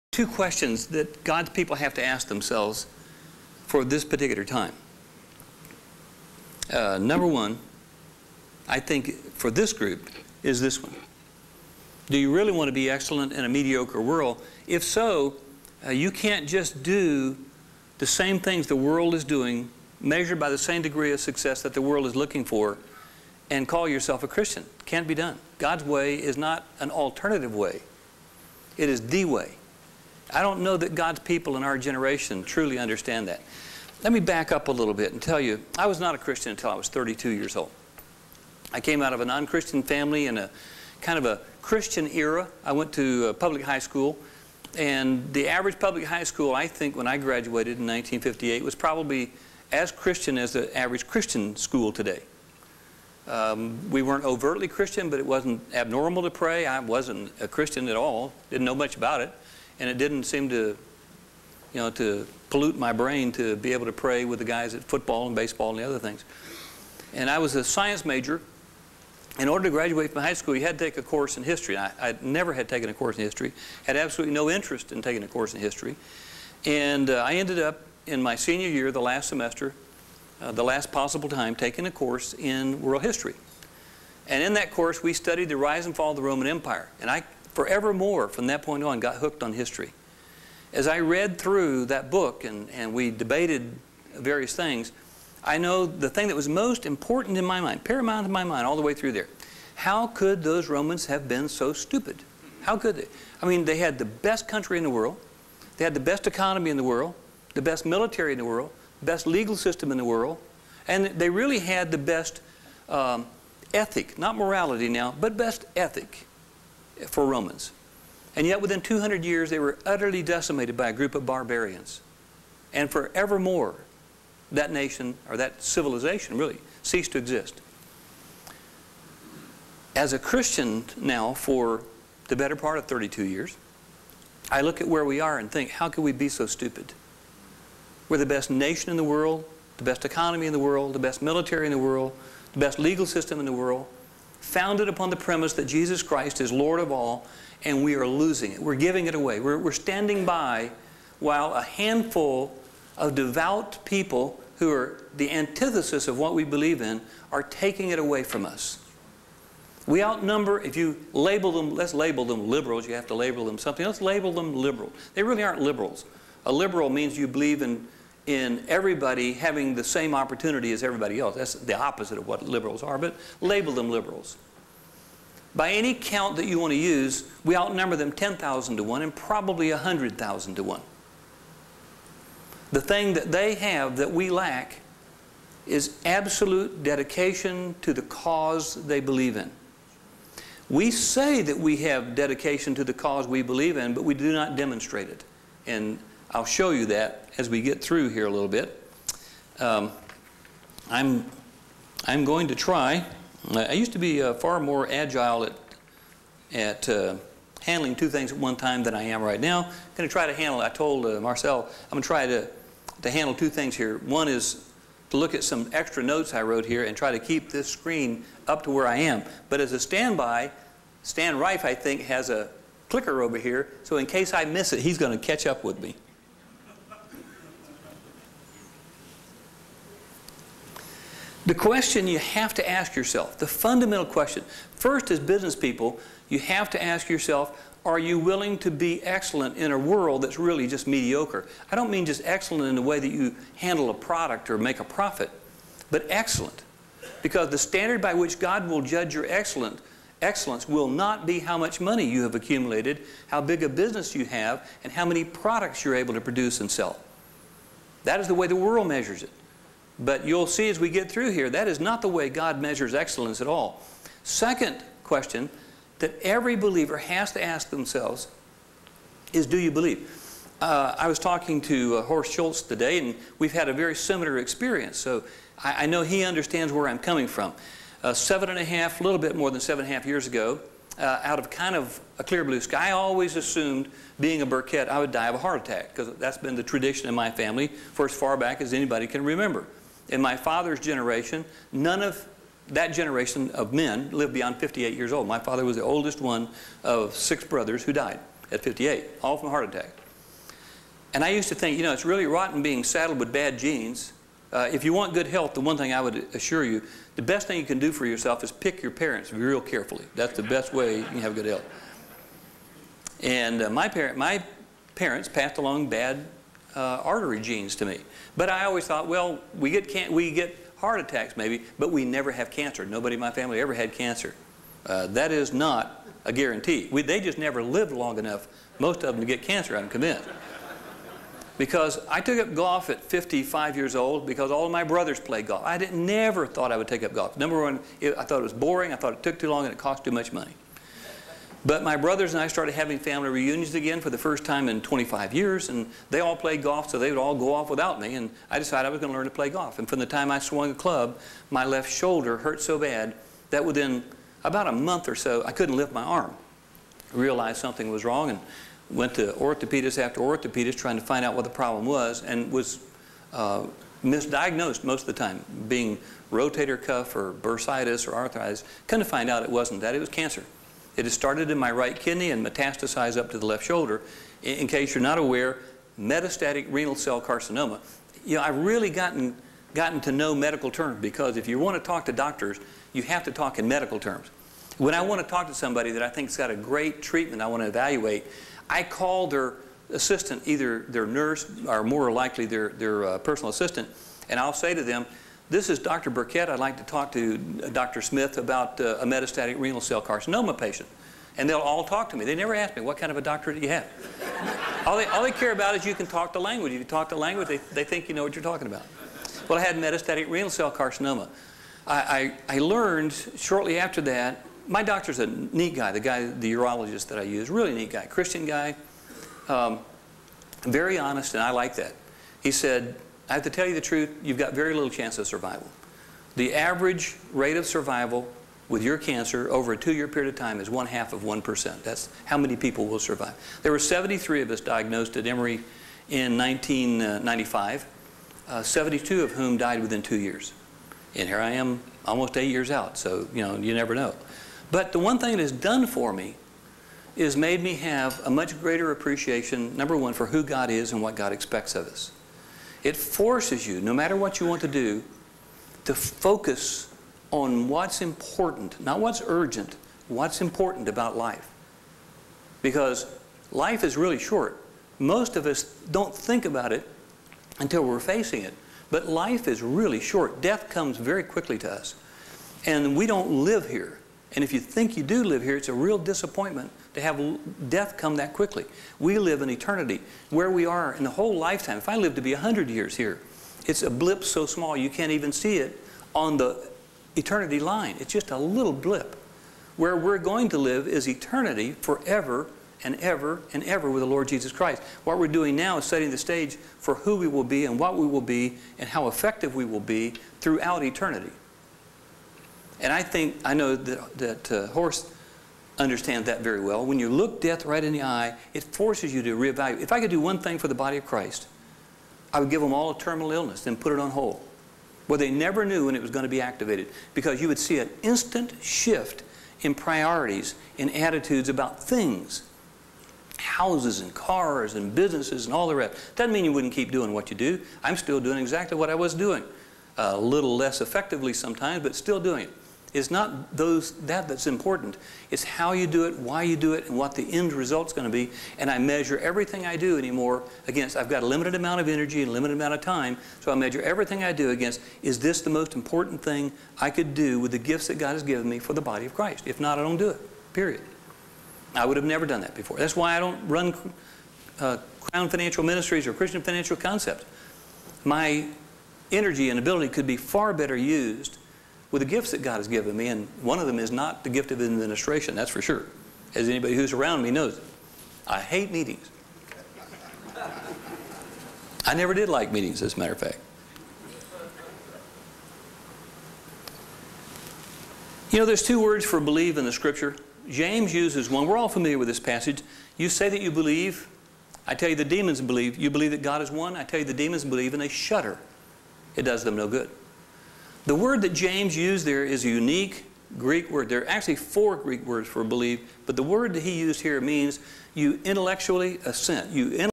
FCCI 2002 Int. Conference--LarryBurkett 1st 10 minutes
Larry Burkett's last public teaching for FCCI. His passion for Christ expressed in the marketplace was never greater than in this talk. Larry challenged Christians to live lives that are distinctive from non-Christians. He also shares that his terminal illness brought many blessings that he desired to impart to others.